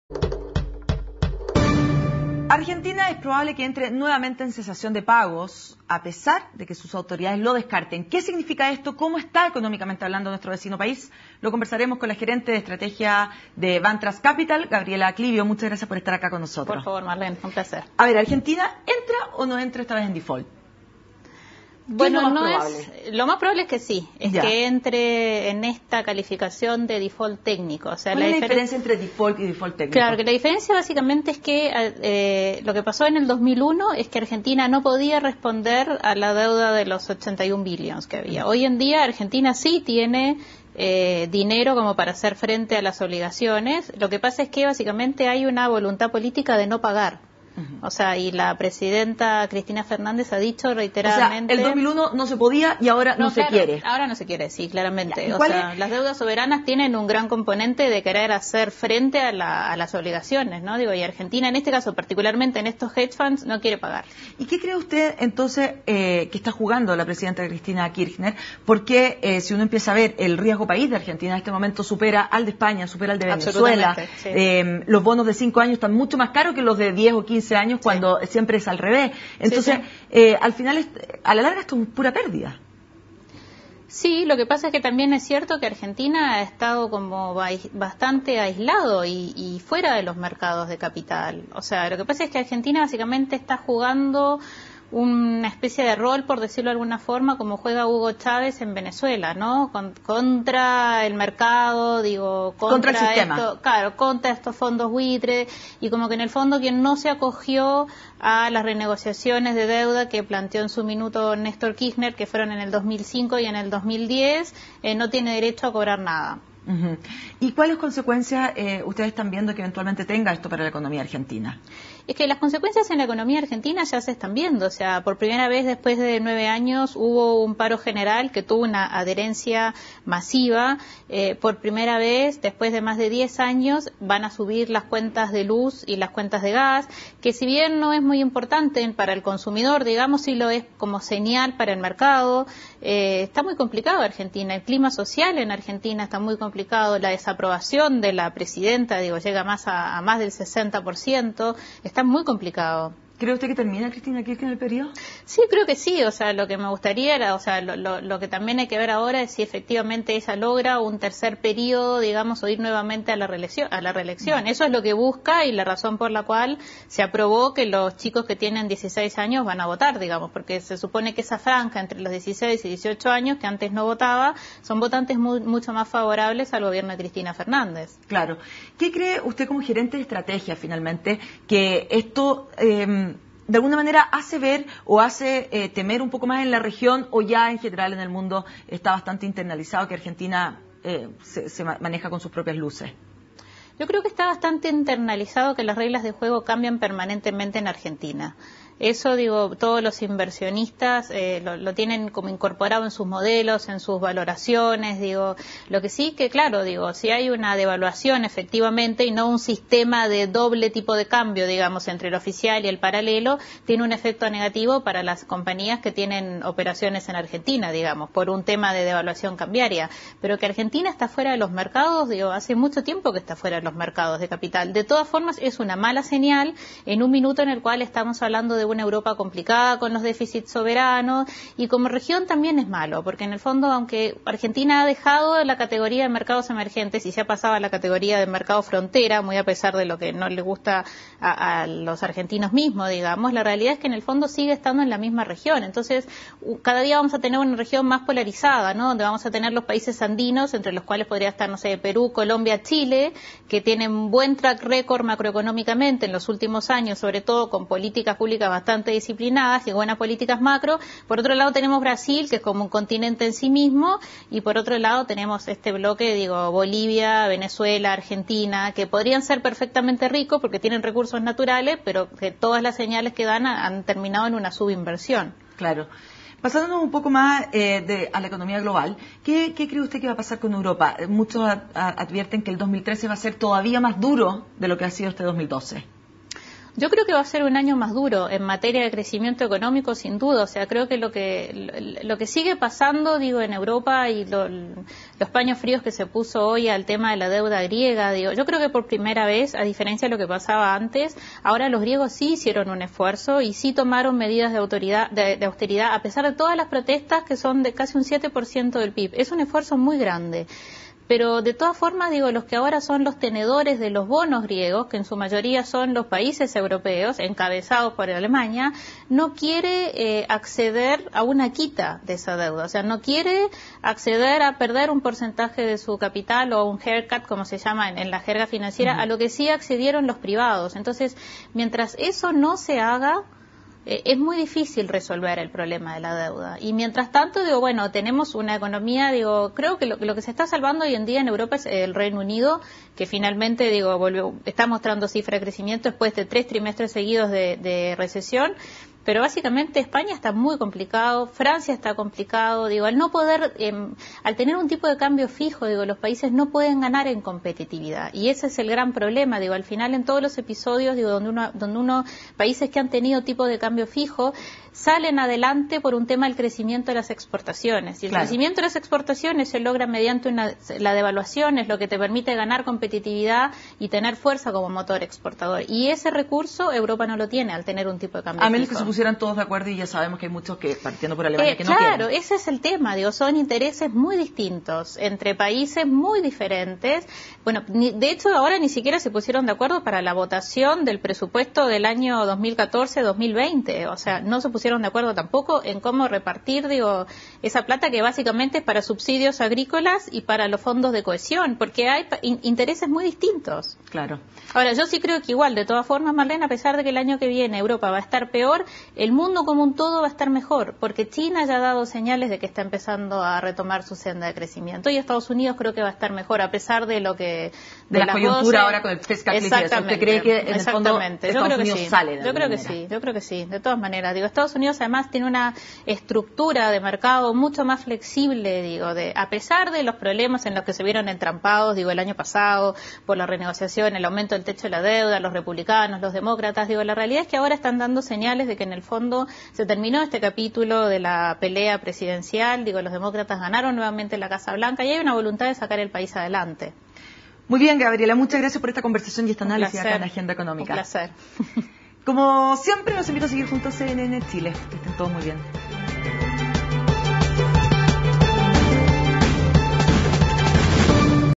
La economía argentina y el escenario de su posible cesación de pagos
El país trasandino arriesga caer nuevamente en un cese de pagos por compromisos del 2001.